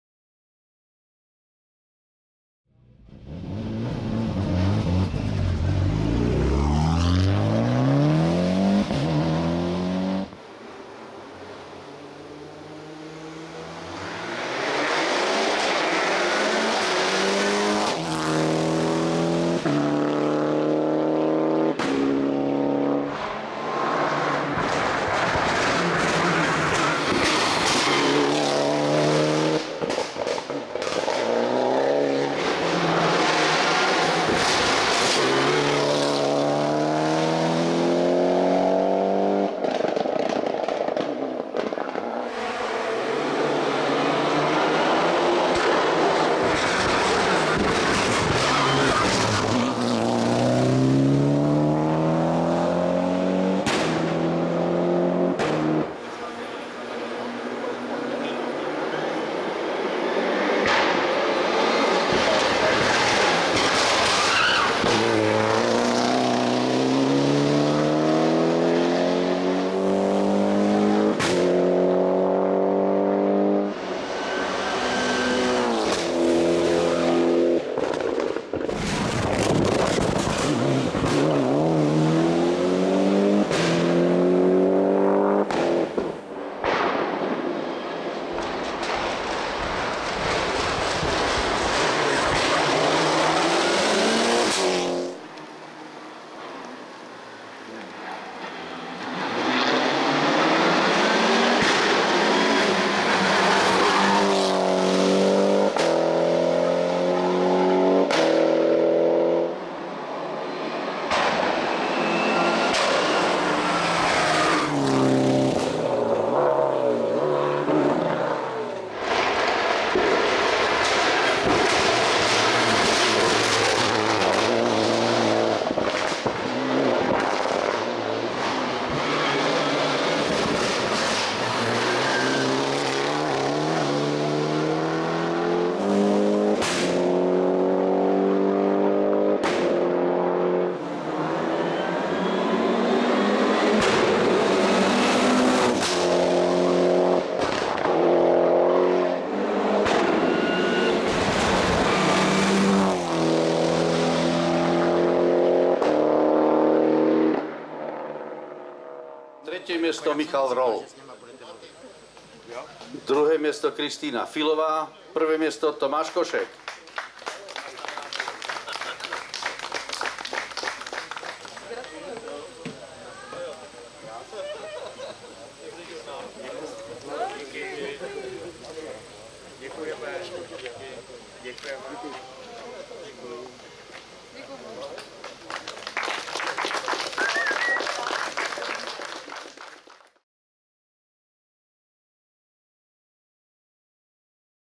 Mitsubishi Lancer Evo X R4 - Michal Roll - VIII.Vrbenský vrch 2021 - Vrbno pod Pradědem
Michal Roll si s vozem Mitsubishi Lancer Evo X R4 dojel pro třetí místo ve třídě E1+2000/4WD v závodech do vrchu VIII.Vrbenský vrch ve Vrbně pod Pradědem  v seriálu Maverick Hill Climb Czech 2021.